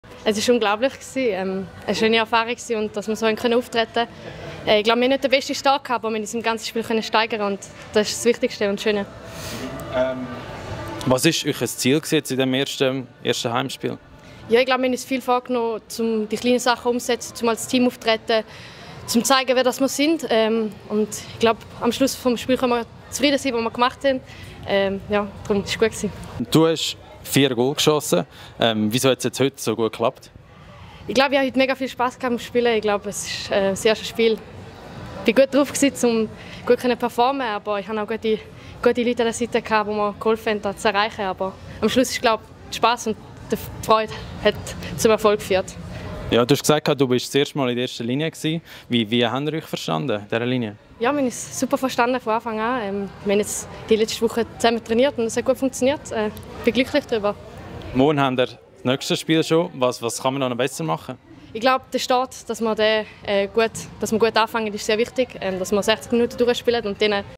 Interview mit Naemi Herzig - EVZ vs Brandis
Der EVZ gewinnt Zuhause gegen Brandis mit 17:0! Naemi Herzig mit der #26 erzählt im Interview, wie ihr erstes Spiel gelaufen ist.